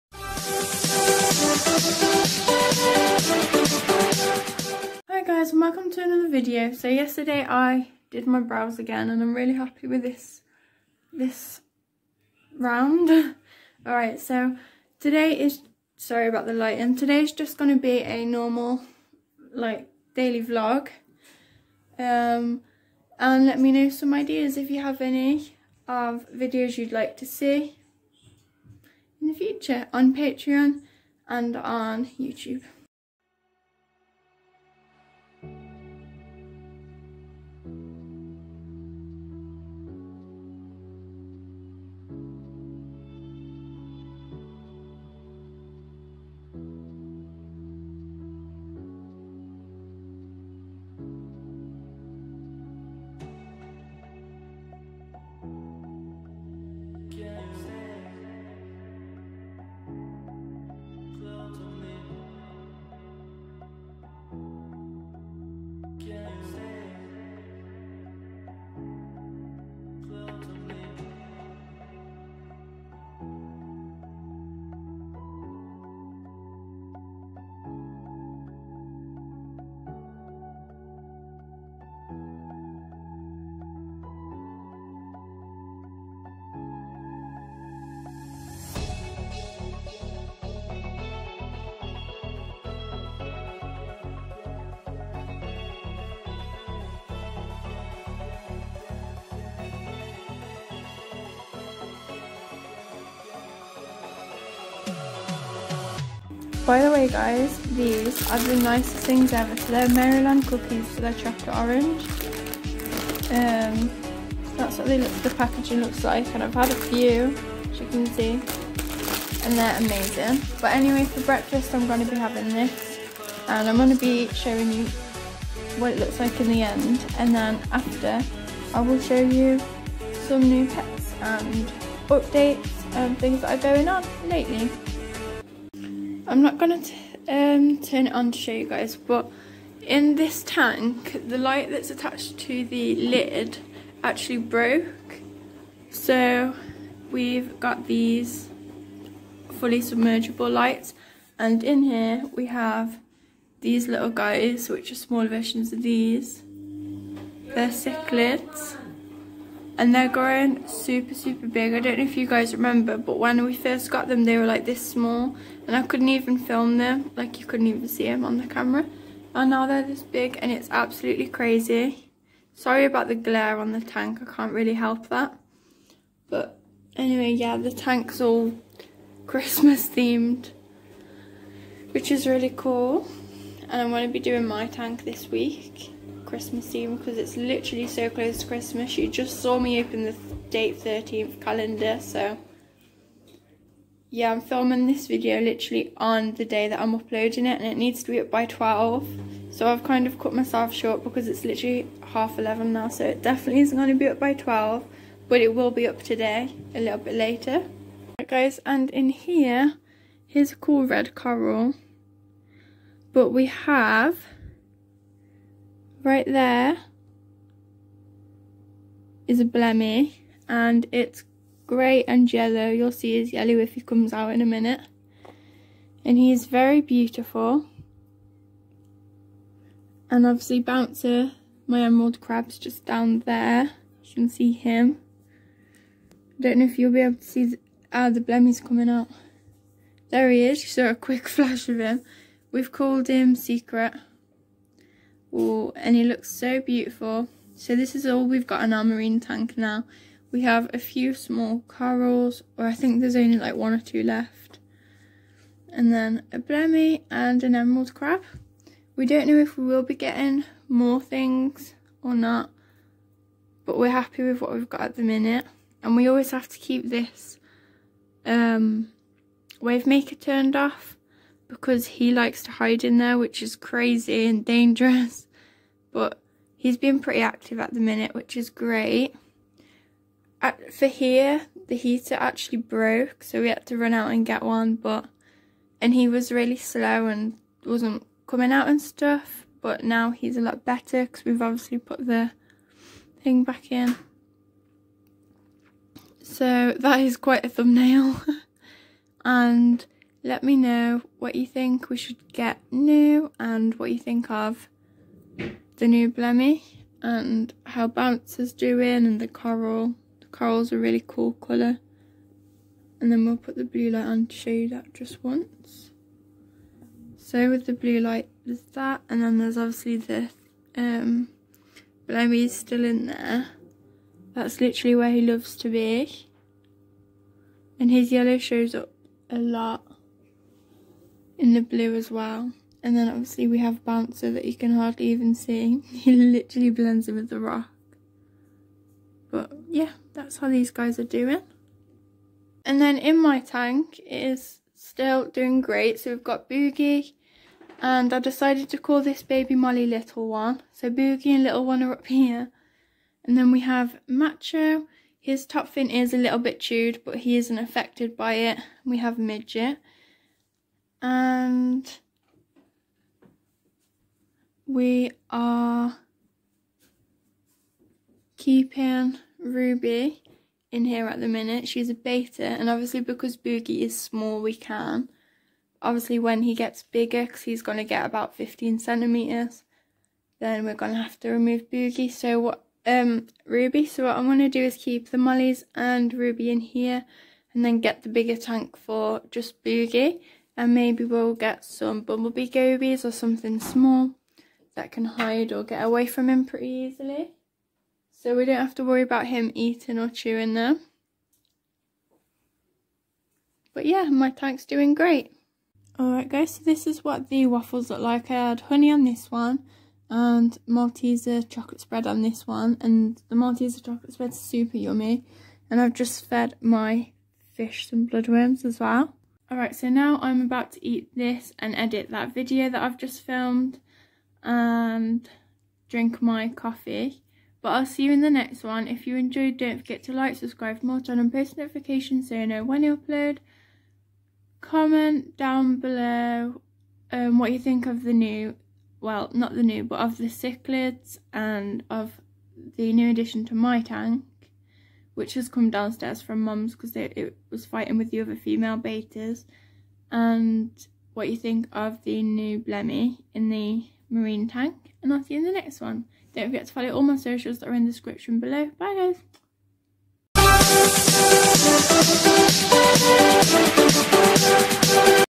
New marine blemmy! Convict cichlid fry is growing!! betta with pufferfish??! vlogmas day 13
amazon wishlist

crystalanne902

crystalanne902

crystalanne_902

depop
crystalannenine